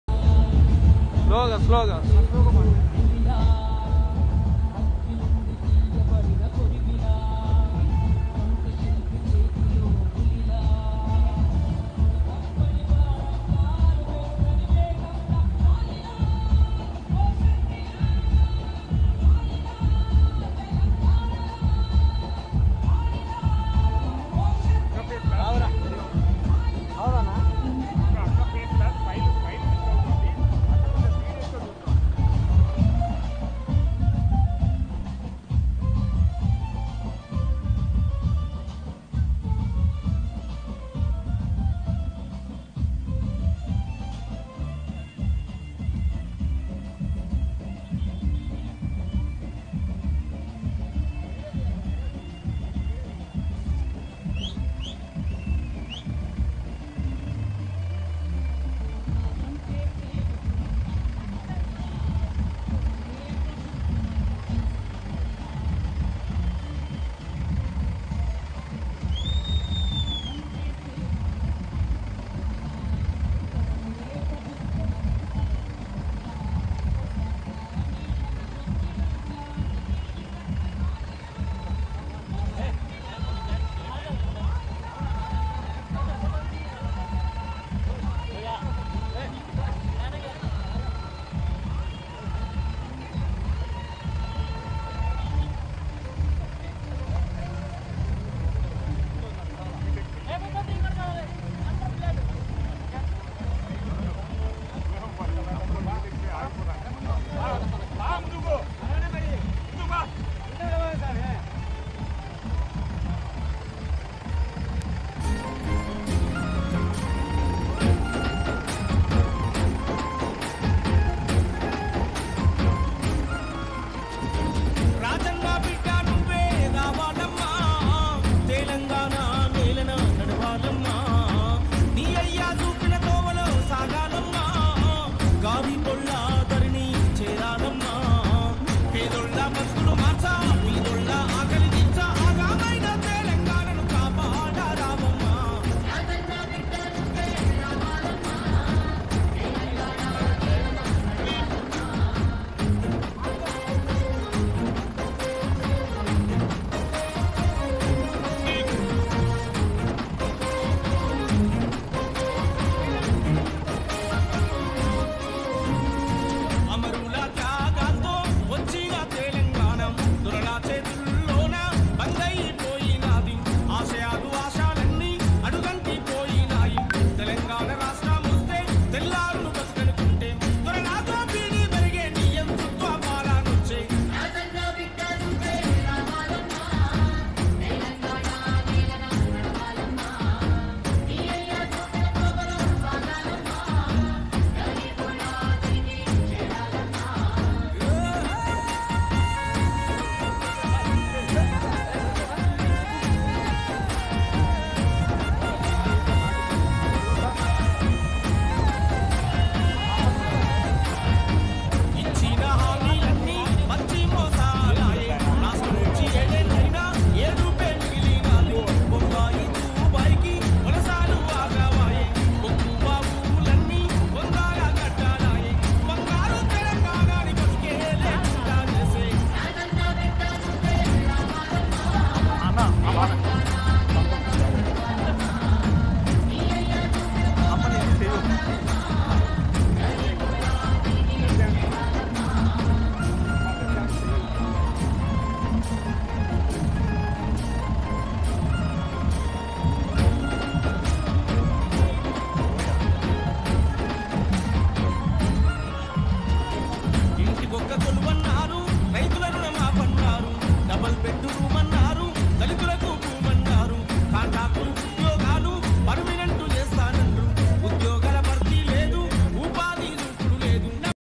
తగ్గేదేలే అంటున్న షర్మిల | YS Sharmila Drives Tractor in Praja Prashtana Yatra | Leo Telangana
Leo Politics

Leo Health

Leo Entertainment

Leo Devotional